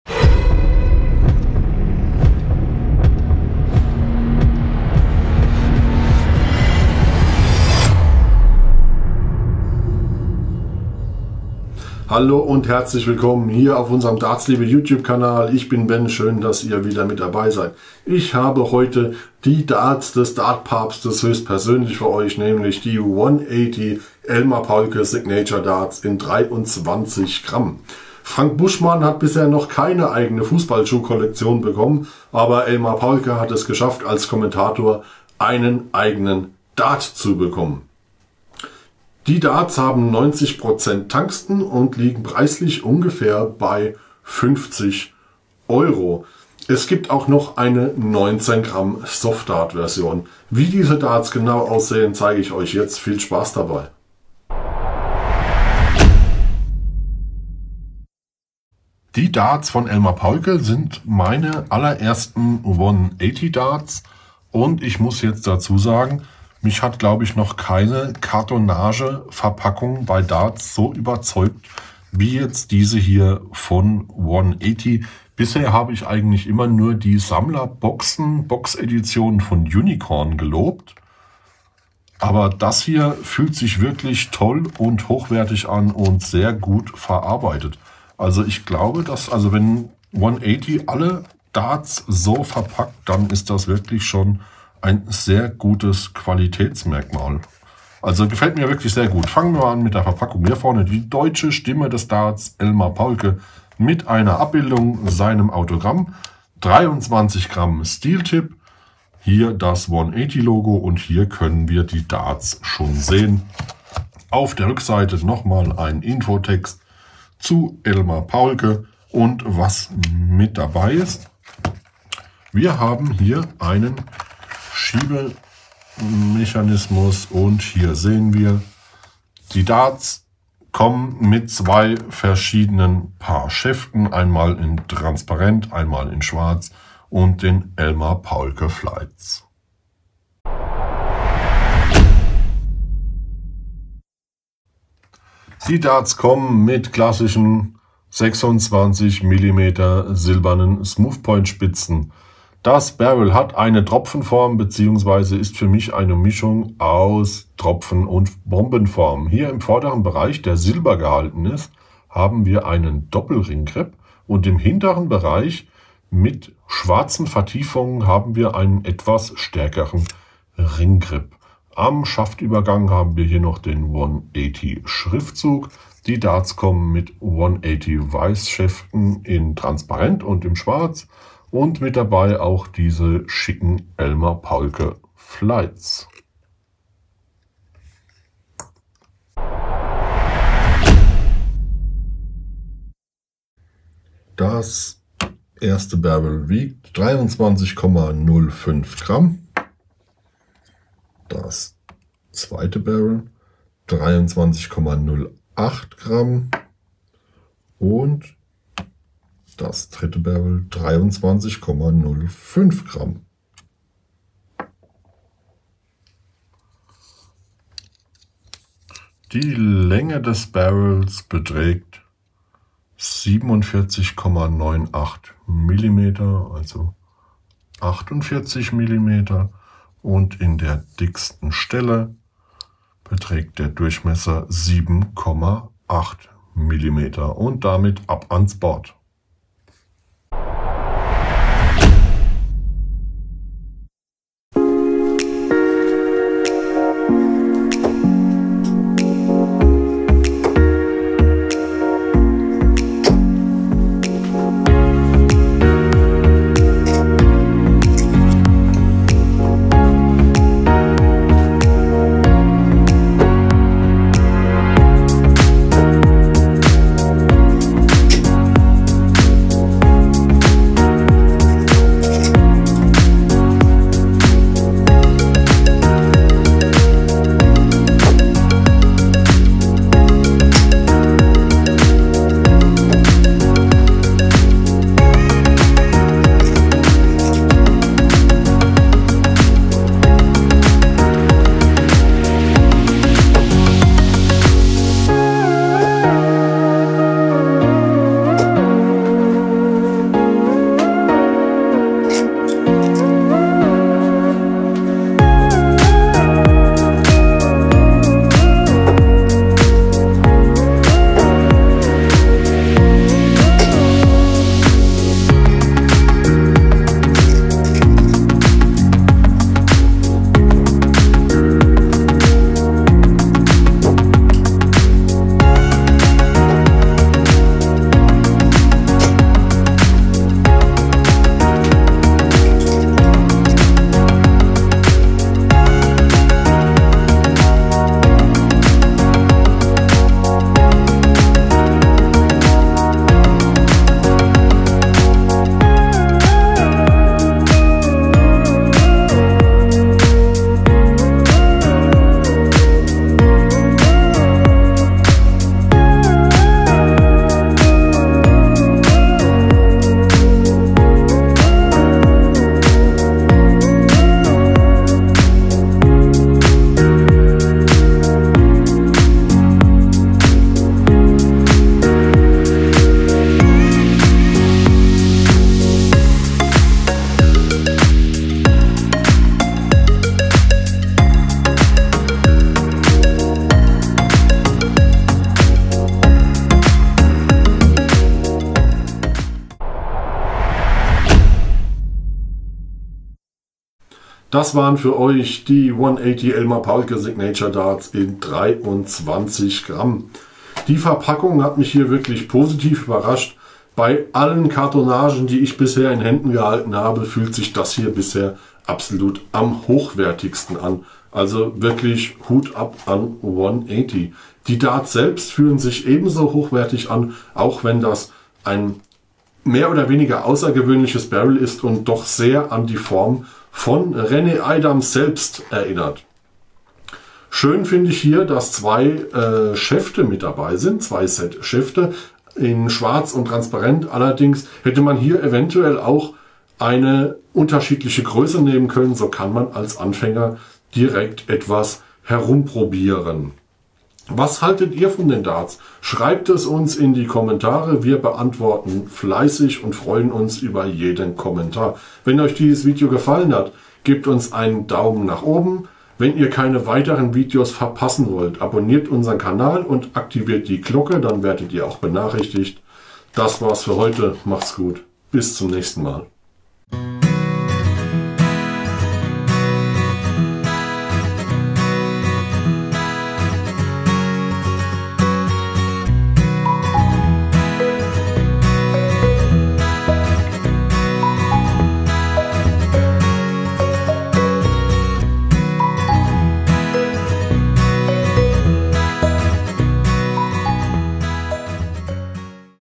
One80 Elmar Paulke | DARTS REVIEW | DARTSLIEBE
Frank Buschmann hat keine eigene Fussballschuh Kollektion, aber Elmar Paule hat seine eigenen Darts! One80 hat die Kommentatorenlegende unter Vertrag genommen. Ben stellt euch seine Darts diesem Video vor.

KAPITEL
0:00 Intro
0:54 Unboxing
2:20 Review
3:09 Messen & Wiegen
4:00 Ab ans Board
6:29 Fazit & Outro